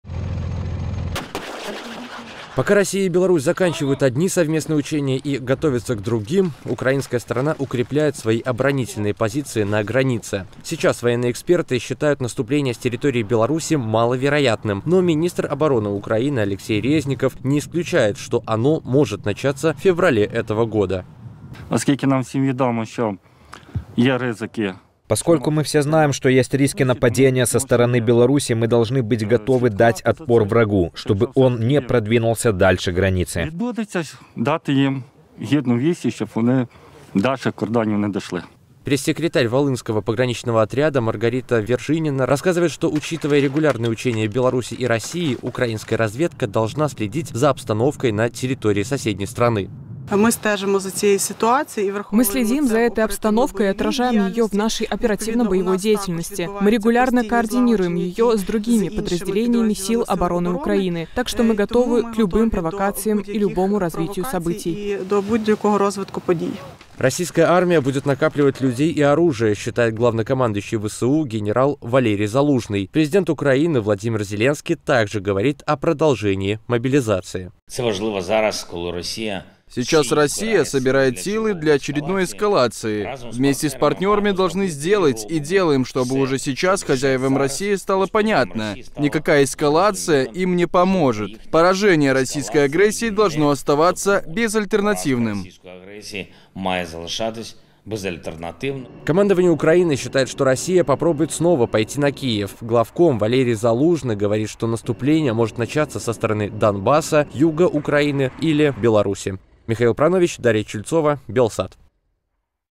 Украина укрепляет позиции на границе с Беларусью / Новости Украины
Украинские военные готовятся к наступление из территории Беларуси.